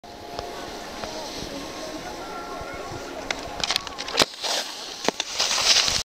スキーボード パンダチャレンジ キッカーツーグラブ しらかば2in1
パンダがしらかば2in1のキッカーでツーグラブに挑戦してみた
　1人目：ニアテールグラブ→チップグラブ（トラックドライバー？）
　2人目：テールグラブ→ミュートグラブ